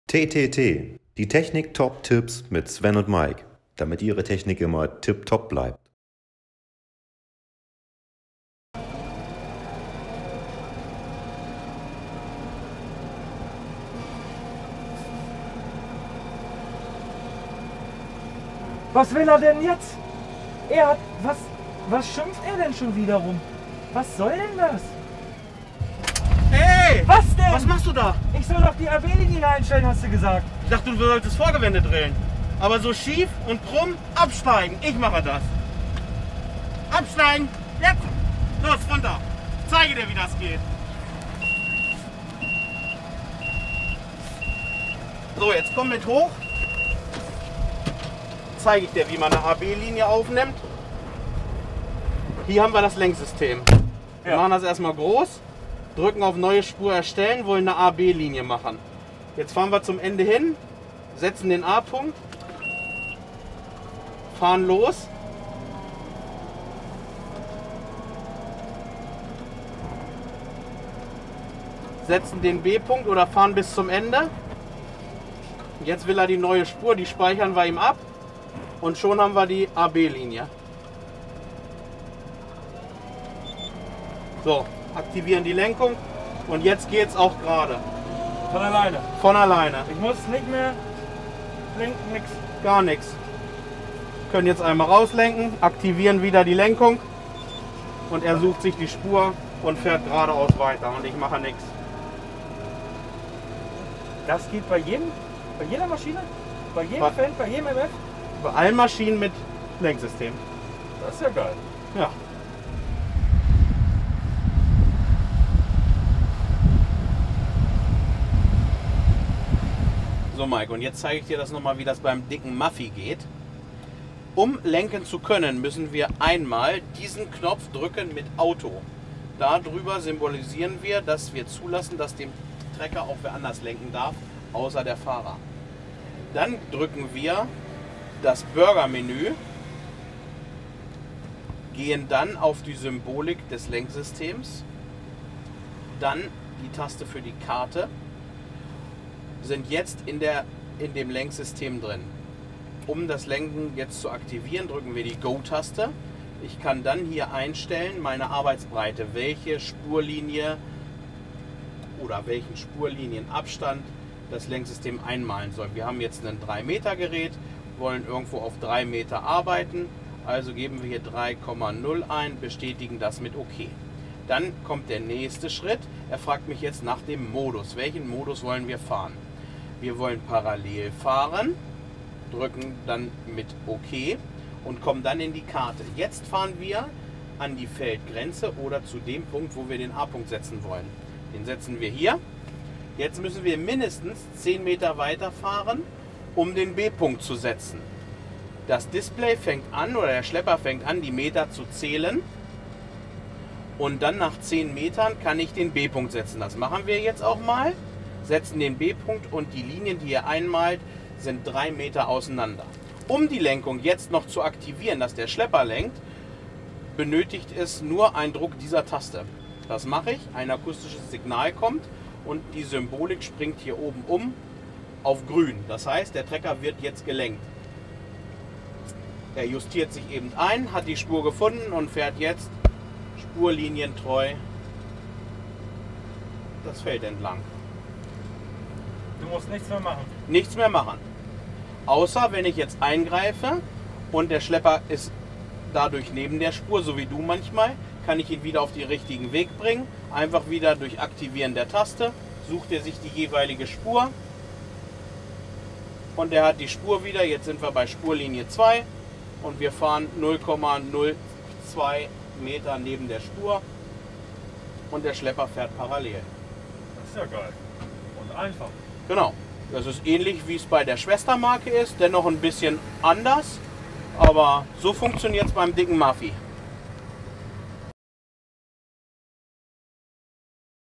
Technik Top Tipps | #034
Neues von den Technik Top Tipps: Sven und Maik erklären Euch die Einstellung von AB-Linien beim Fendt und Massey Ferguson für die optimale Spurführung 💪